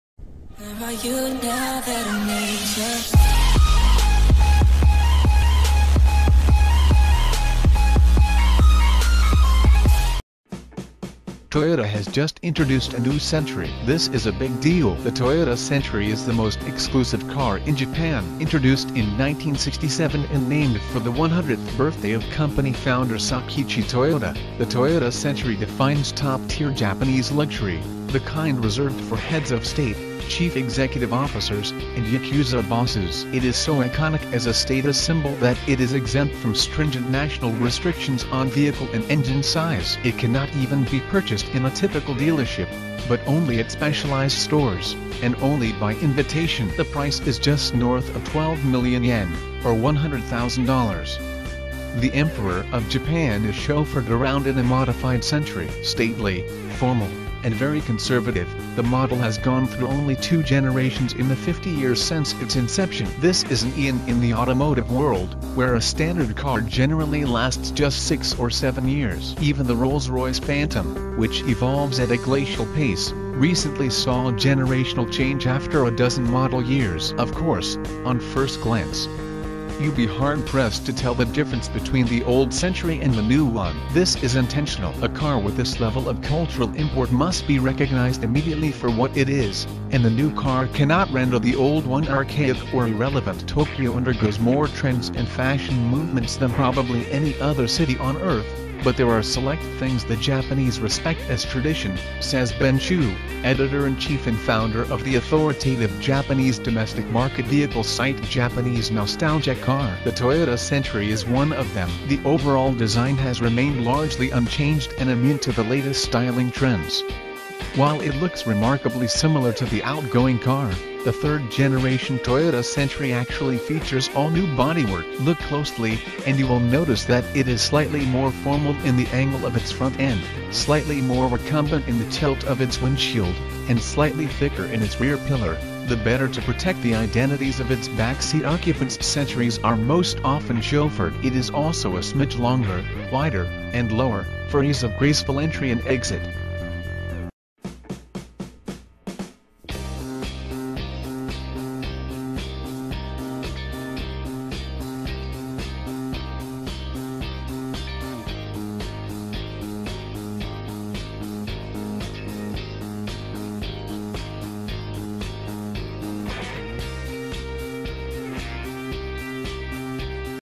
Toyota Review - Toyota's Rolls-Royce for half the money - the incredible 2018 Century - CarBest
Toyota Review - Toyota's Rolls-Royce for half the money - the incredible 2018 Century.
Toyota has just introduced a new Century. This is a big deal. The Toyota Century is the most exclusive car in Japan. Introduced in 1967 and named for the 100th birthday of company founder Sakichi Toyoda, the Toyota Century defines top-tier Japanese luxury, the kind reserved for heads of state, chief executive officers, and Yakuza bosses. It is so iconic as a status symbol that it is exempt from stringent national restrictions on vehicle and engine size. It cannot even be purchased in a typical dealership, but only at specialized stores, and only by invitation. (The price is just north of 12 million yen, or $100,000.) The emperor of Japan is chauffeured around in a modified Century.
Stately, formal, and very conservative, the model has gone through only two generations in the 50 years since its inception. This is an eon in the automotive world, where a standard car generally lasts just six or seven years. Even the Rolls-Royce Phantom, which evolves at a glacial pace, recently saw generational change after a dozen model years.
Of course, on first glance. you’d be hard pressed to tell the difference between the old Century and the new one. This is intentional. A car with this level of cultural import must be recognized immediately for what it is, and the new car cannot render the old one archaic or irrelevant.
“Tokyo undergoes more trends and fashion movements than probably any [other] city on earth, but there are select things the Japanese respect as tradition,” says Ben Hsu, editor in chief and founder of the authoritative Japanese domestic-market vehicle site Japanese Nostalgic Car. “The Toyota Century is one of them. The overall design has remained largely unchanged and immune to the latest styling trends.”
While it looks remarkably similar to the outgoing car, the third-generation Toyota Century actually features all-new bodywork. Look closely, and you will notice that it is slightly more formal in the angle of its front end, slightly more recumbent in the tilt of its windshield, and slightly thicker in its rear pillar, the better to protect the identities of its back-seat occupants (Centuries are most often chauffeured). It is also a smidge longer, wider, and lower, for ease of graceful entry and exit.

Thank's For Watching My Video About Toyota Review - Toyota's Rolls-Royce for half the money - the incredible 2018 Century, i hope you enjoy and don't forget for share, like and subscibe my channel...

CarBest